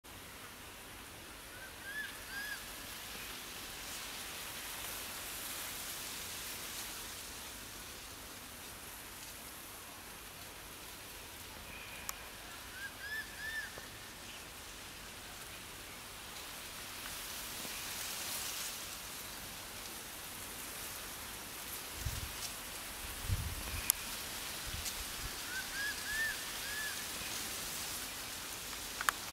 蛇鵰鳴叫 Calling of Crested Serpent Eagle
蛇鵰 Crested Serpent Eagle, Spilornis cheela
50-74cm
Common resident in Hong Kong